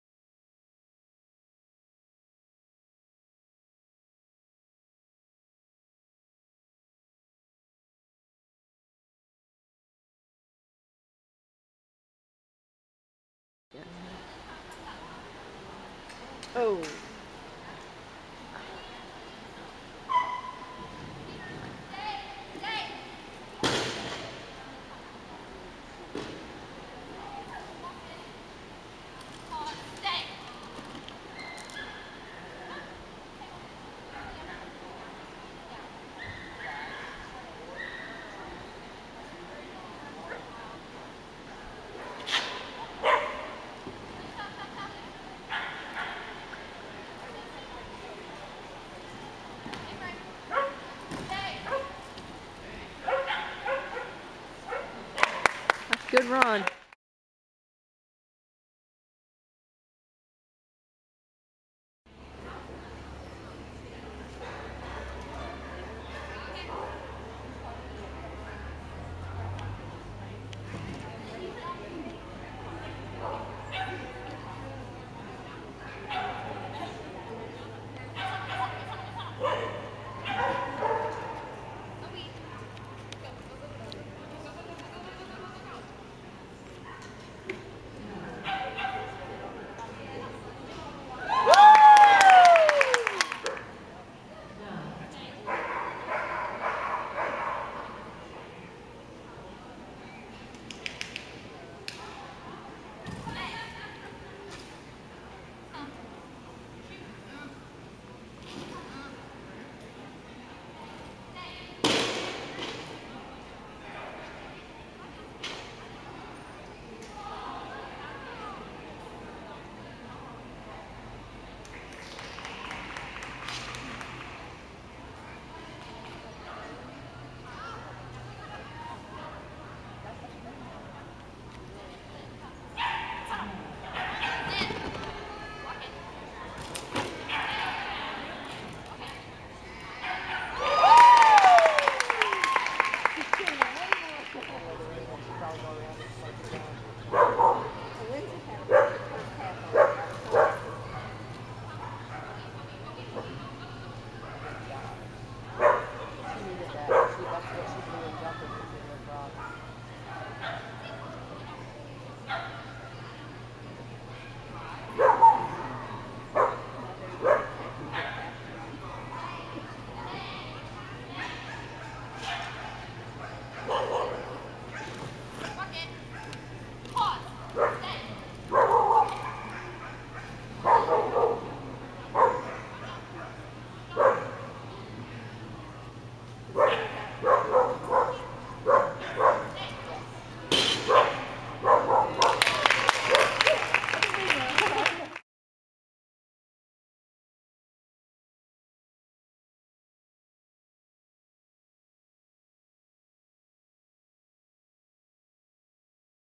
Adrenalyn DAKC May 2010
I'm so incredibly happy to have Lyn sound again. She's been dying locked up on house arrest for the last two months.

Two days after we returned from Tulsa she came up lame in the left rear...re-injured her groin. Shortly after she came up lame on her right front with a torn pec. -- They think those two injuries may have occurred simultaneously because the front tear was a lateral injury. Possibly from a slip in the weaves? --
After both of those seemed to be back to 100% my mom began slowly working her while I was at school. Only a day or so after, she came up lame on her rear left...again. This time when we took her in they mentioned it may be an ACL. After going to the specialist, he informed us it wasn't the ACL or the groin...it was her 2 weight bearing toes in that foot. For about a month we've been rehabbing the toes, and she seems to be doing fantastically.

This was a fabulous weekend for her first trial in 2 months. She really gives it everything she has for every single run.